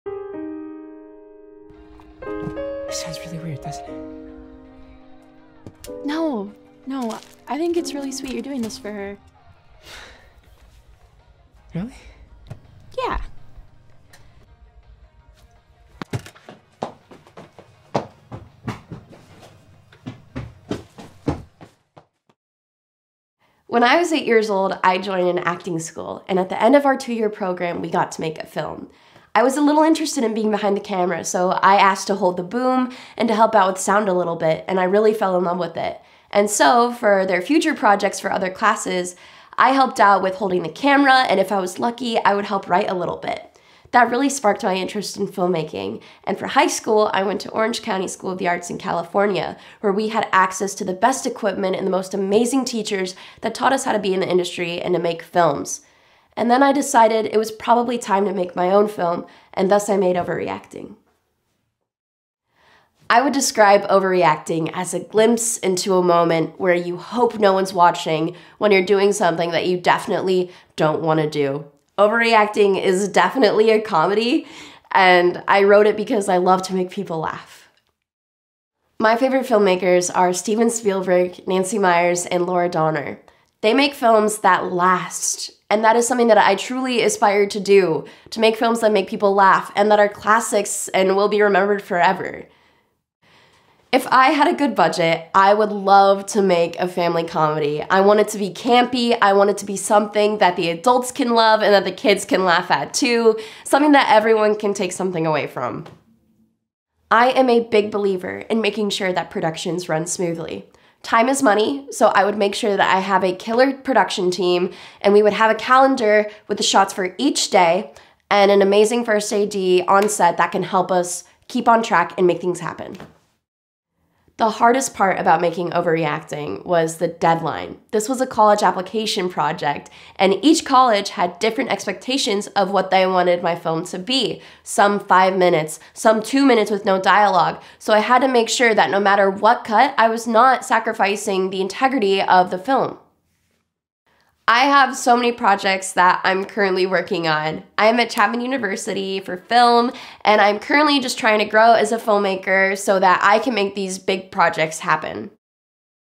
Ovaryacting: An Interview with Jordan Held
A young man must face his fears to make a personal purchase.